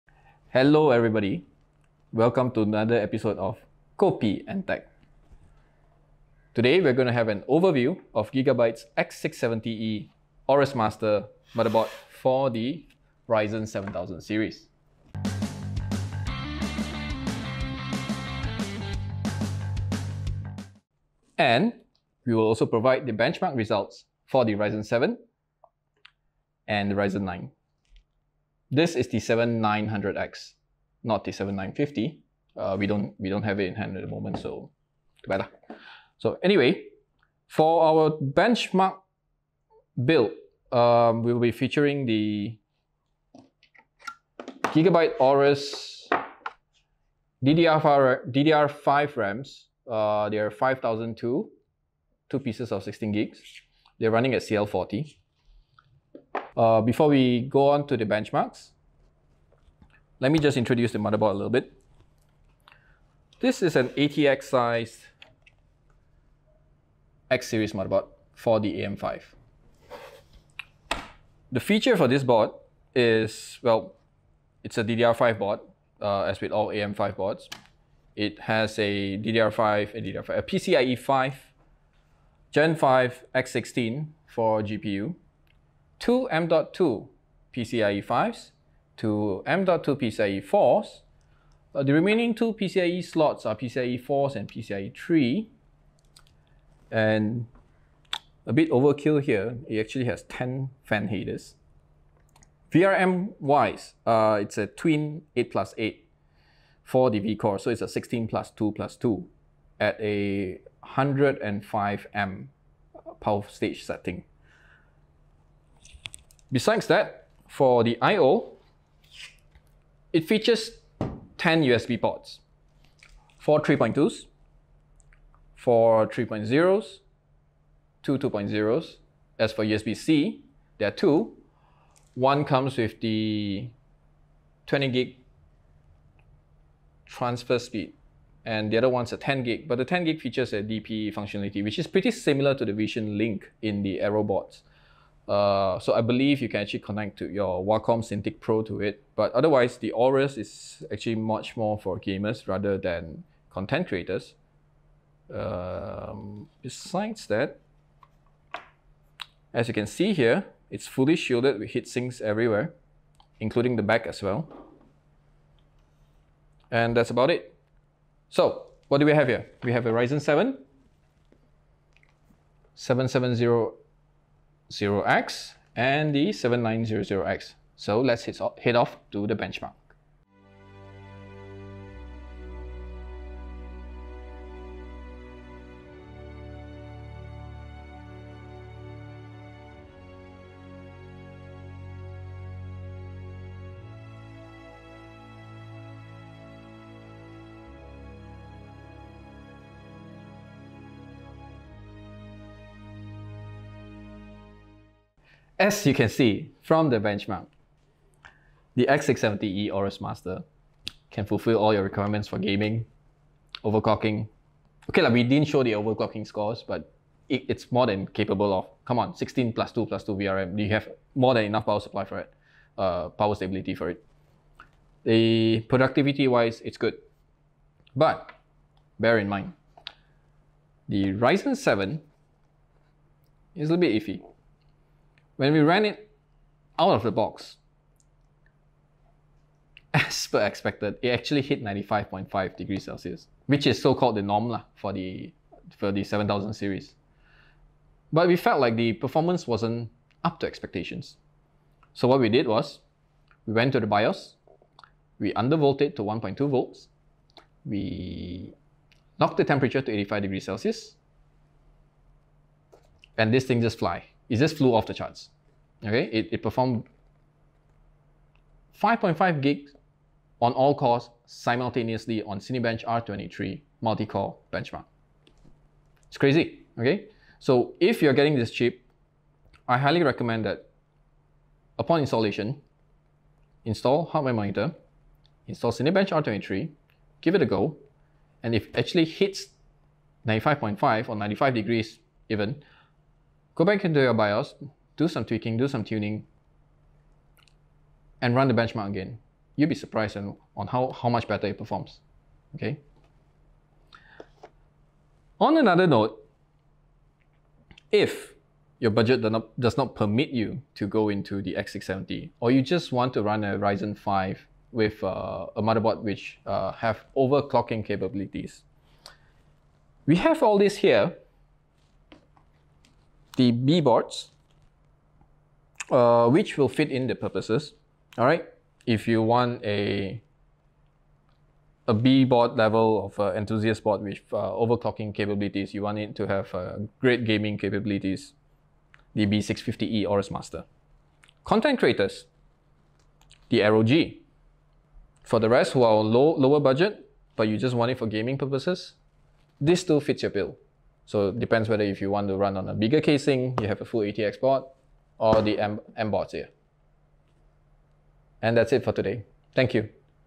K&T EP7 : Gigabyte X670E Aorus Master Overview
A Complete Overview Of The Gigabyte X670E Aorus Master And Performance Of The AMD Ryzen 7 7700X and Ryzen 9 7900X.